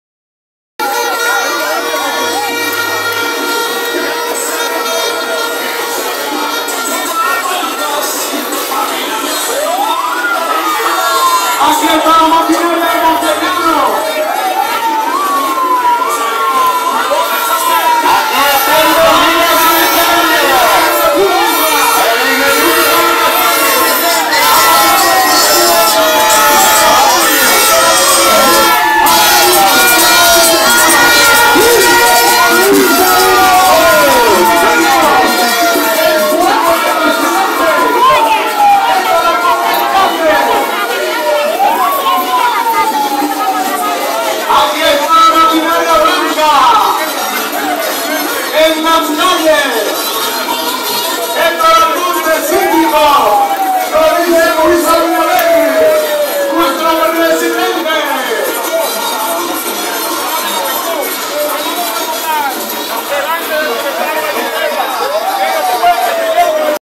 Euforia de Perremeistas en Montellano/ Dicen el cambio va, Abinader 4 mas
Video de Radio Señal Digital